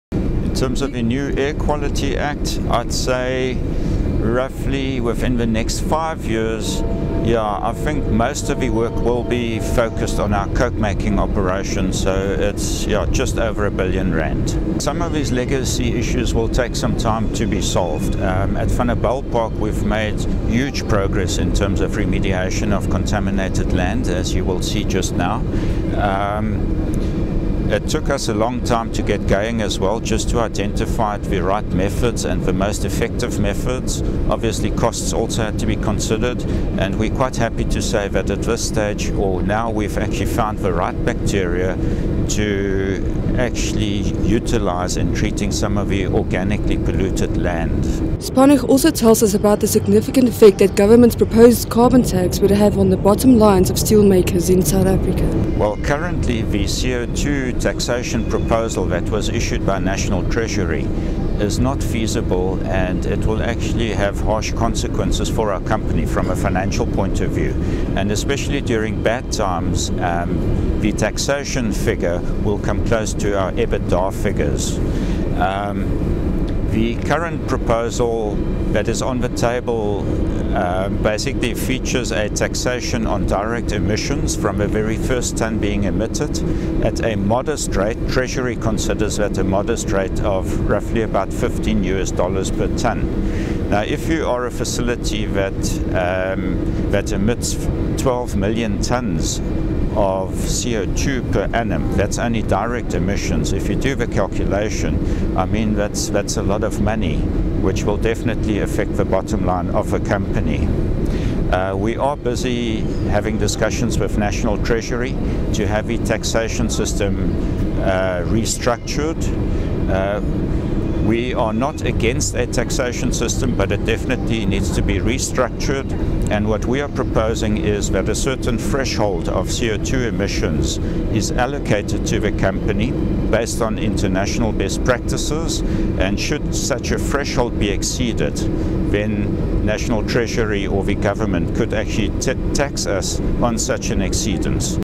Mittal sets aside billions to meet SA's tightening enviro limits
JSE-listed steelmaker ArcelorMittal South Africa (Mittal) would spend more than R2-billion over the coming four years to ensure that all its operations were compliant with the country's increasingly stringent environmental legislation.